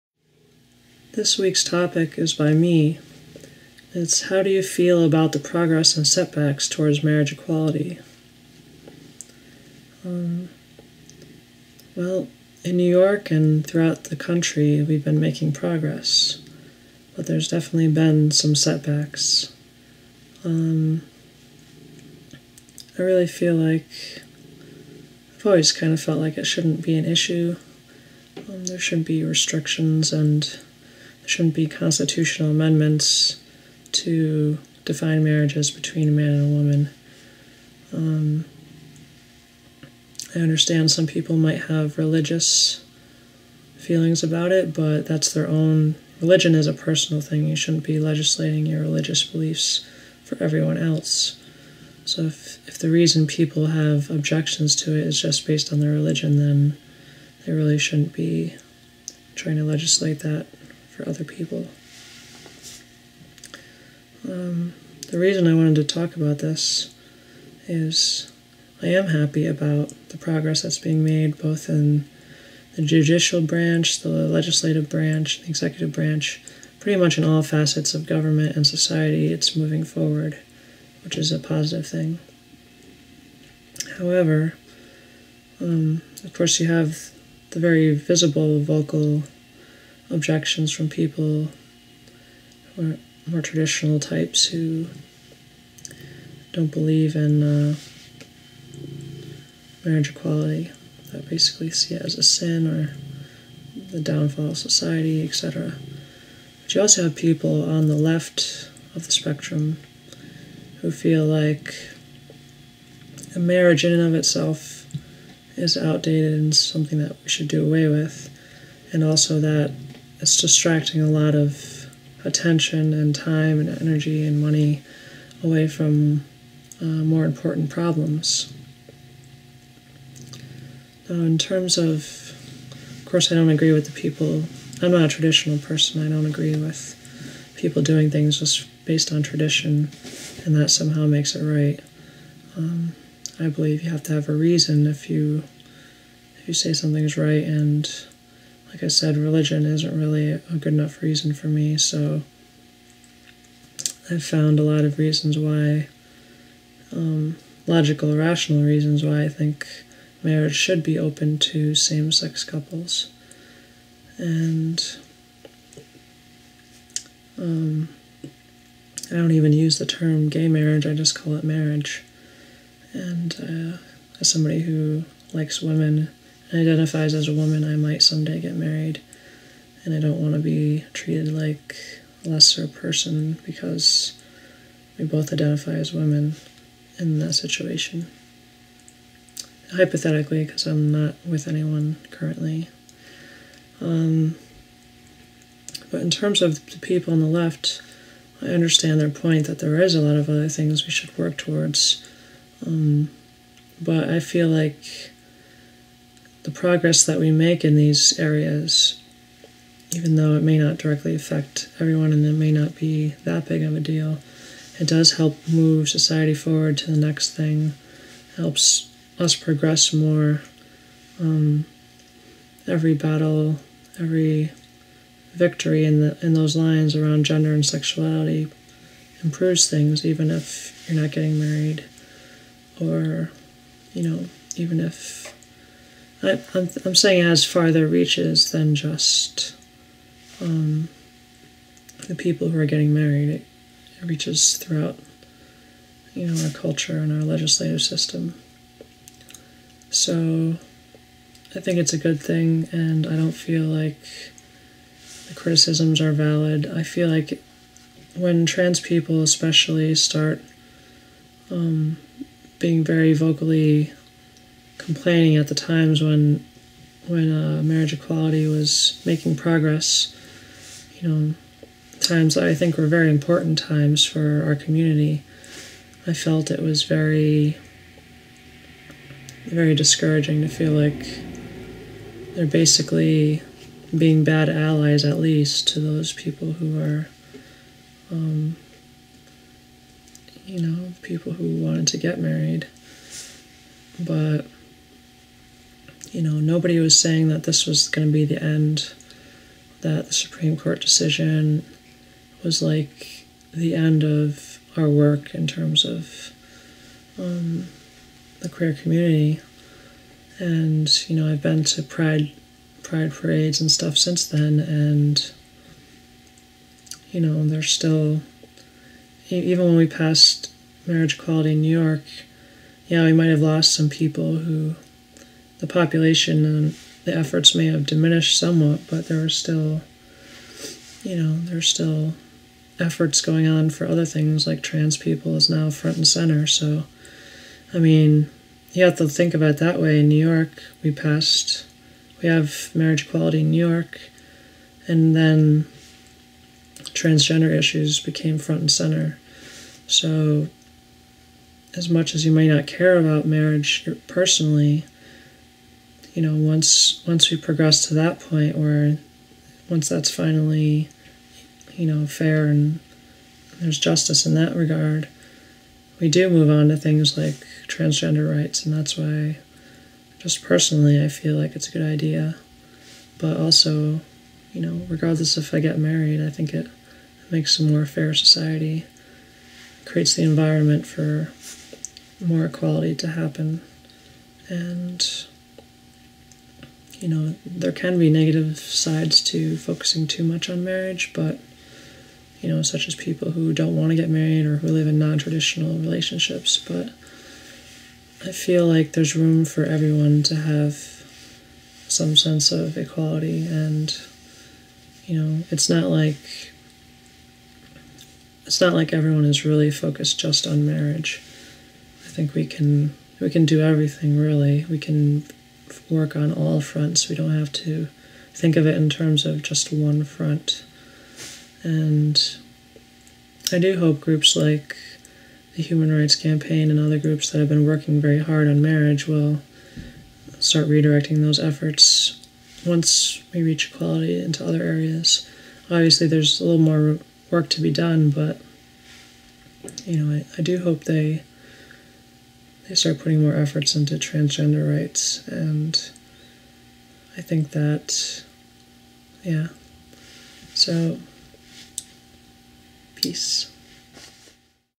TSG 250 - Marriage Equality
Alana: How do you feel about the progress and setbacks toward mariage equality?